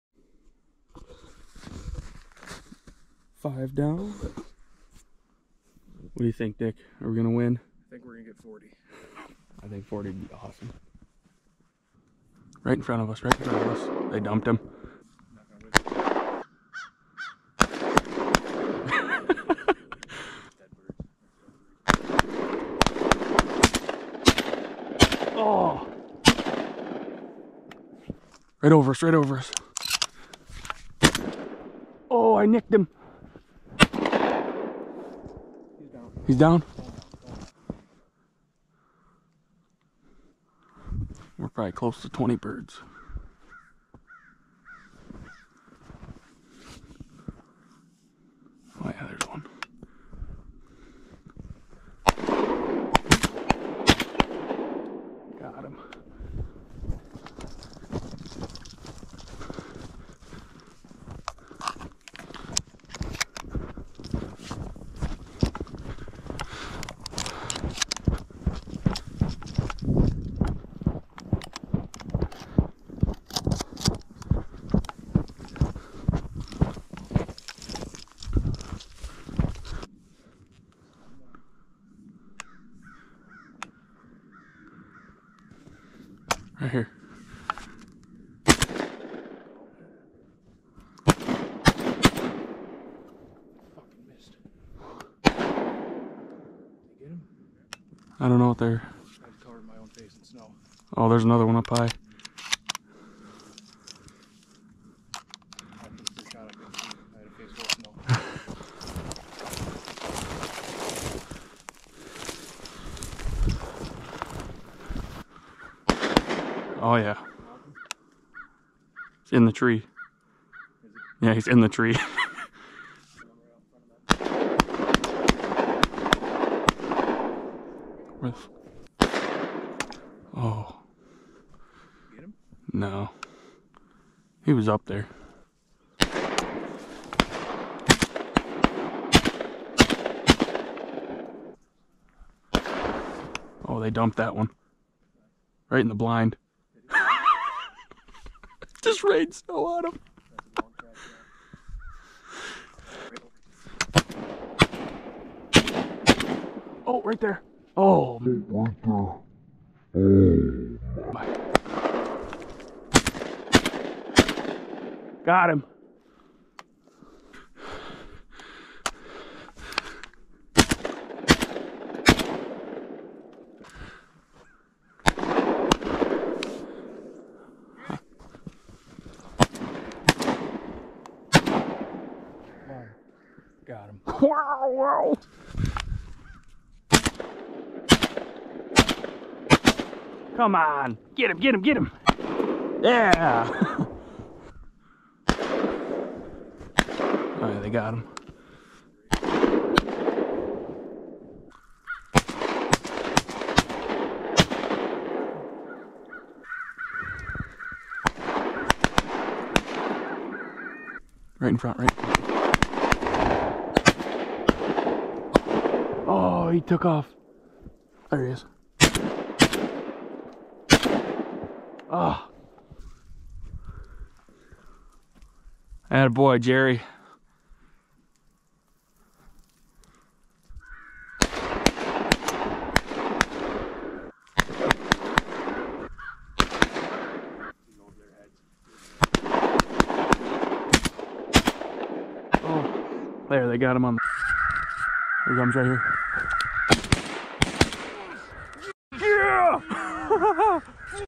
Crazy February Crow Shoot With My Remington V3 Pro
Footage from back in February on our first tourney of the year, and I shot extra bad. We ended up breaking our record for birds in a hunt and got first.